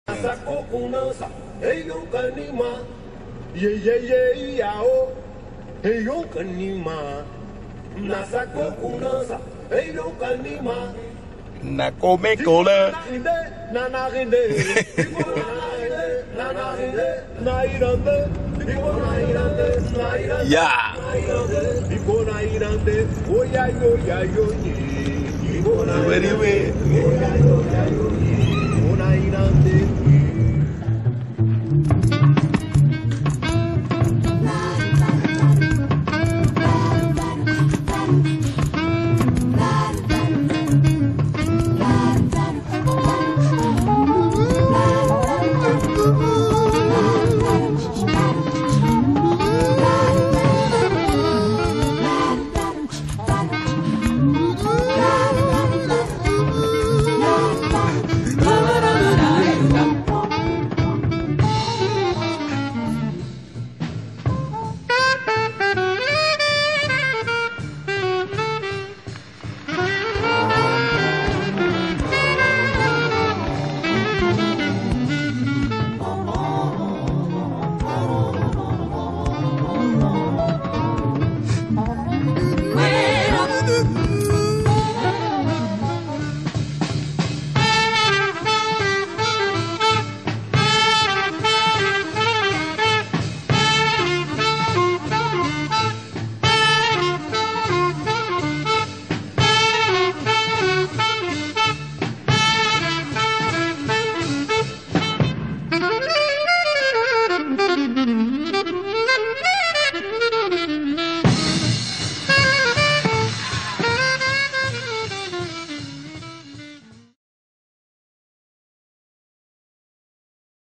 Crónica de um Africano em Lisboa
Teaser do documentário sobre o Zé da Guiné